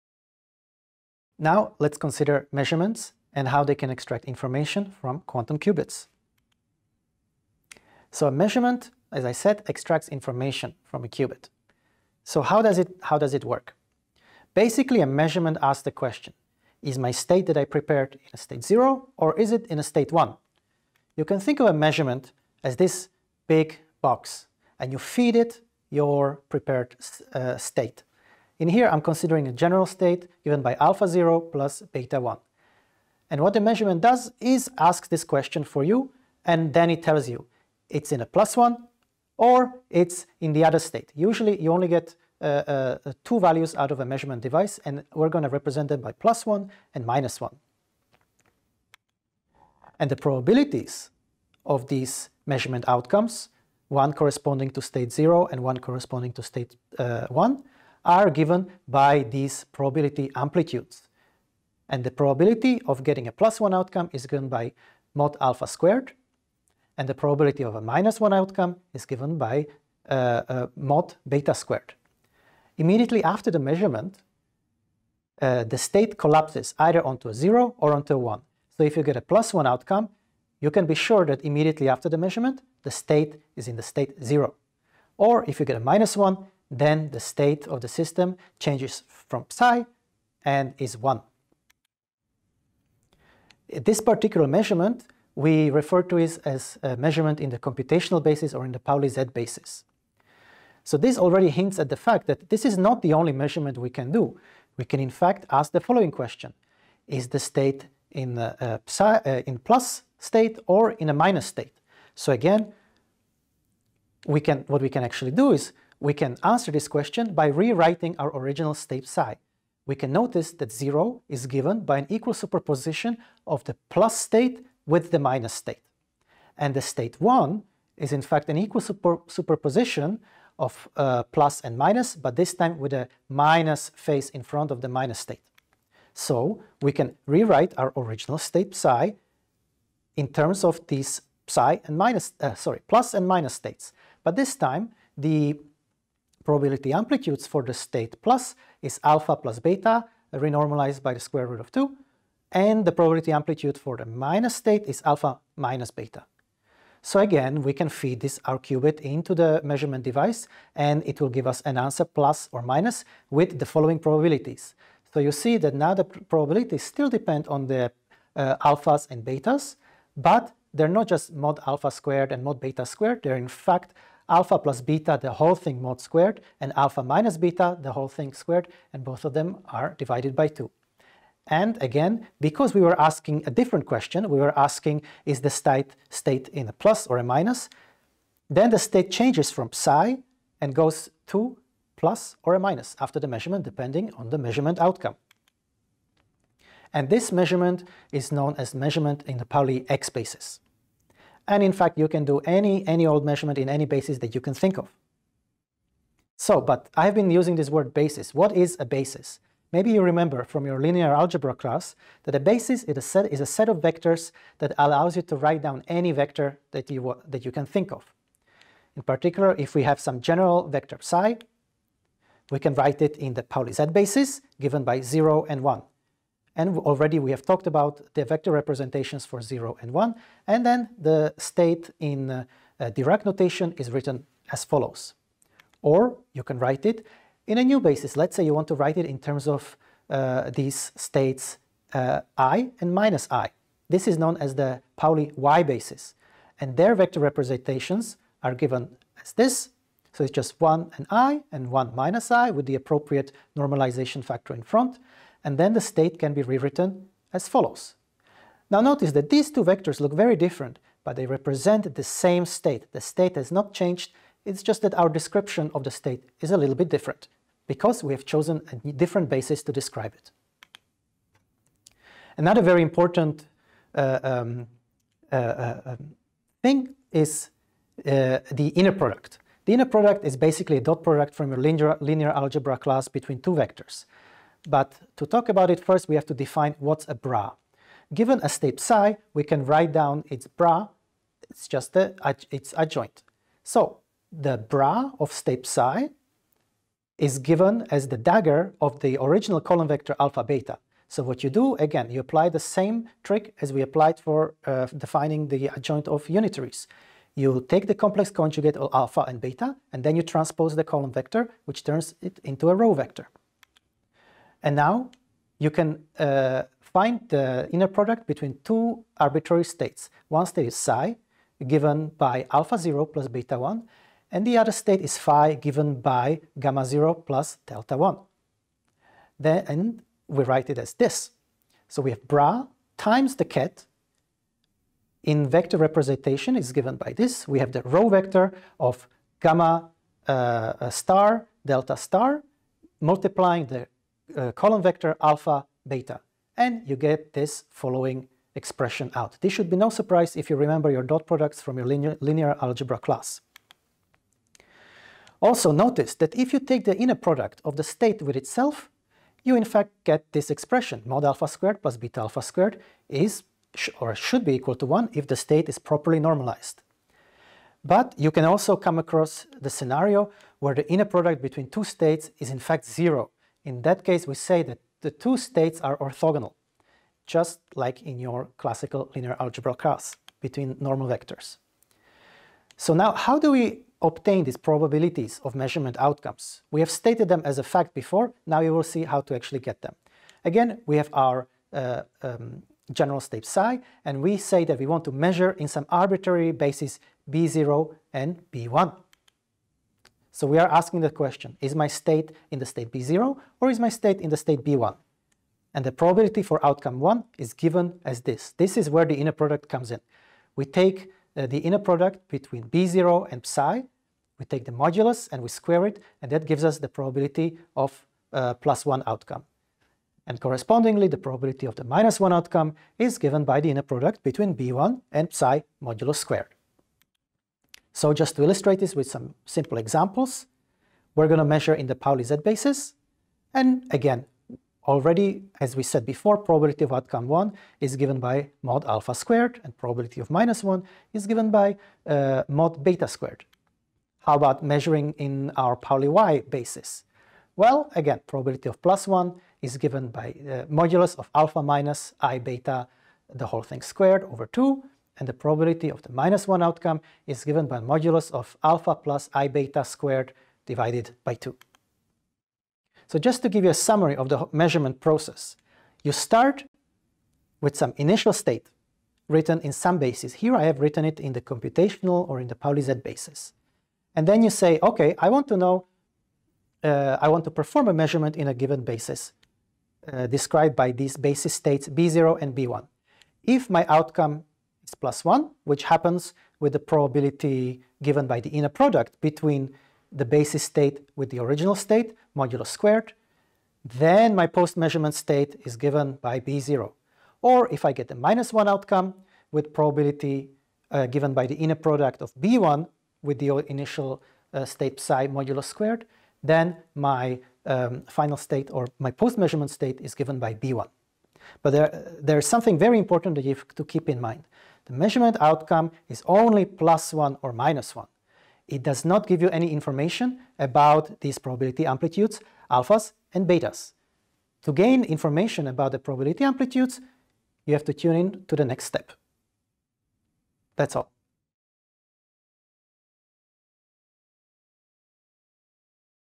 2-3 Measurement
Lesson 2 Quantum States

Step 3: Measurement
We look at how information stored in qubits can be accessed via the measurement process.

This is part of our module: "Overview of Quantum Communications".